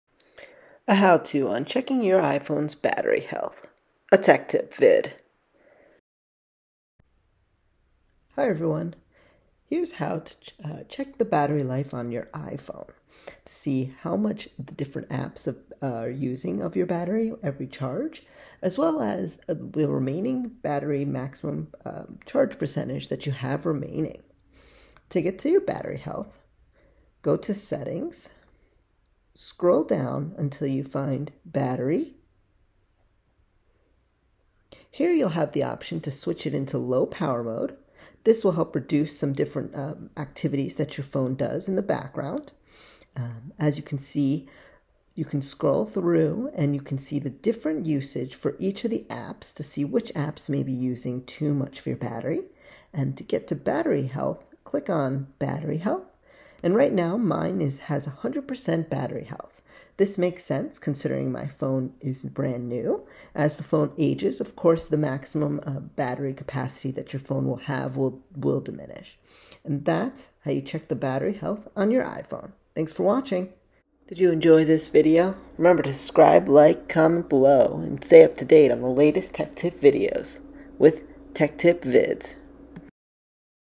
Checking iPhone Battery Health
Battery health deteriorating? No problem! Watch this simple & quick method on checking your iPhone battery health and delete those unnecessary battery consuming apps.